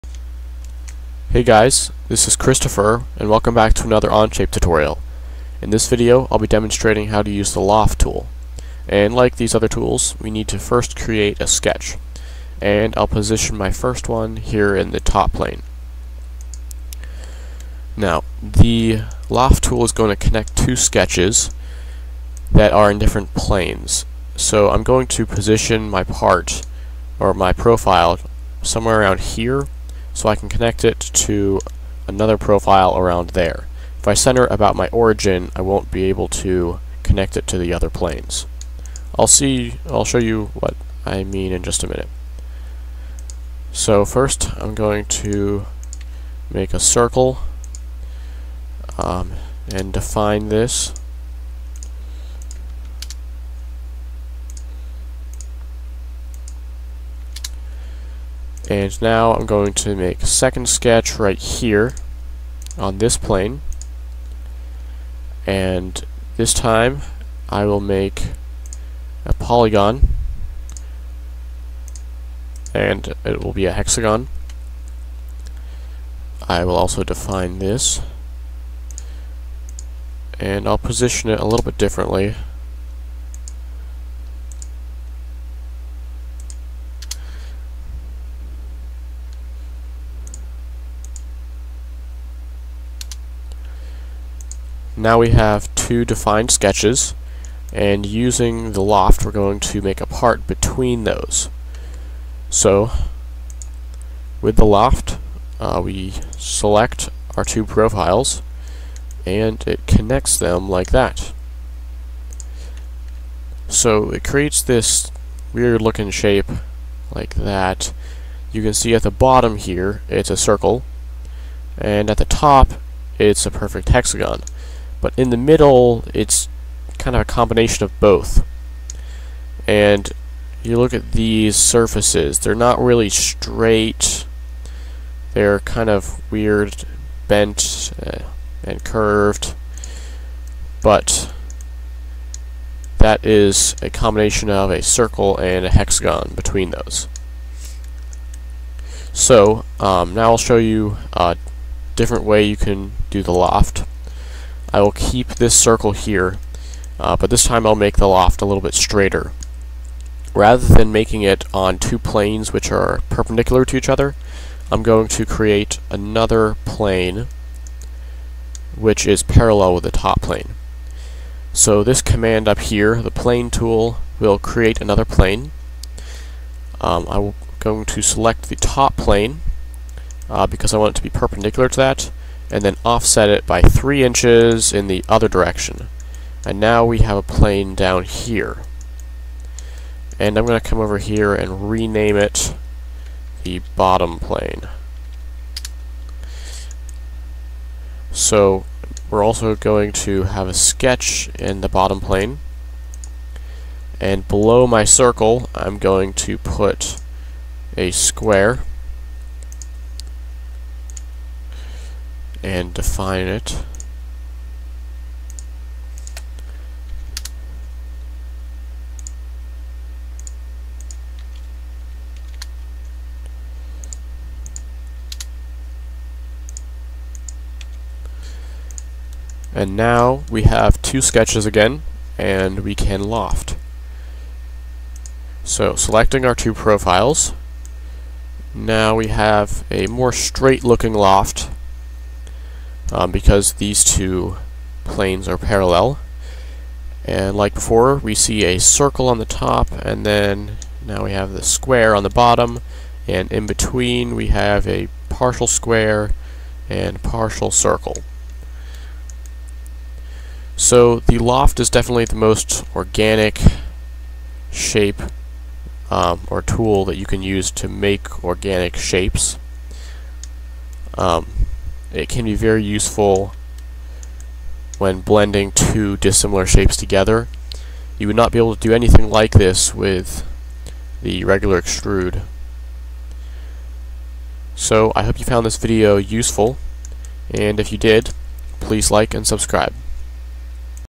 Learn Onshape: #6 Loft - Tutotorial
This is a tutorial about how to use the loft tool in Onshape